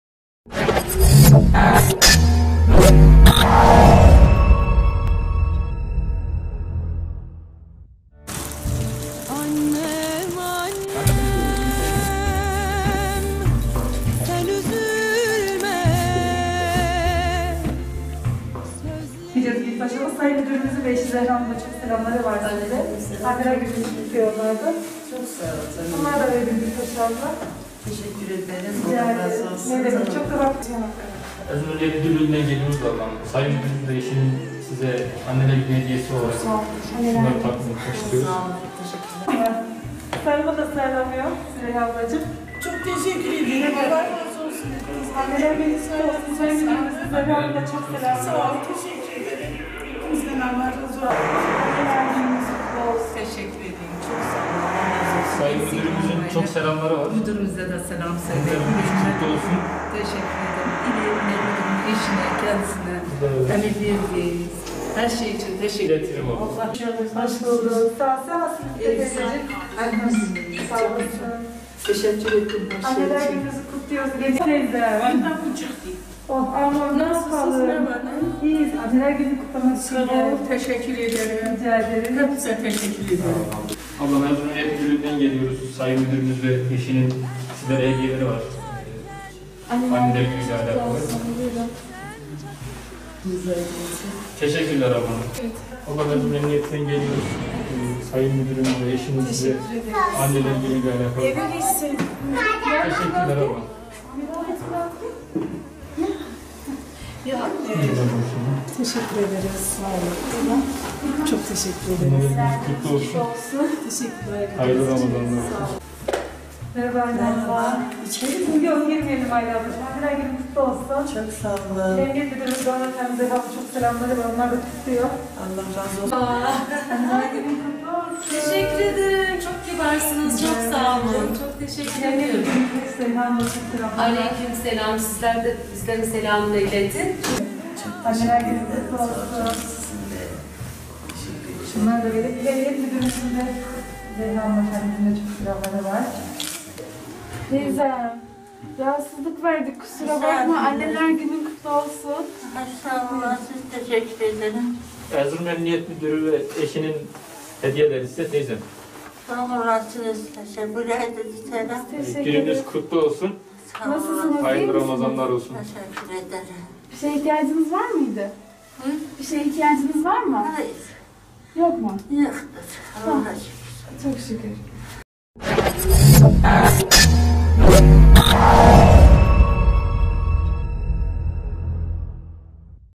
Erzurum polisinden duygulandıran Anneler Günü ziyaretleri
Erzurum'da polis ekipleri, Anneler Günü dolayısıyla kentteki şehit anneleri ile eşlerini ziyaret etti.

Sosyal Hizmetler ve Sağlık Şube Müdürlüğü ile Toplum Destekli Polislik Şube Müdürlüğü personeli, kentteki bazı şehit anneleri ile eşlerinin evlerine giderek Anneler Günü'nü kutladı.

Ziyaretlerde, İl Emniyet Müdürü Mehmet Aslan ile eşinin selamını ileten polisler, şehit ailelerine çiçek ve çeşitli hediyeler takdim etti.

Duygu dolu anların yaşandığı ziyaretlerde, şehit anneleri ile eşleri bu özel günde kendilerini unutmayan polislere teşekkür etti.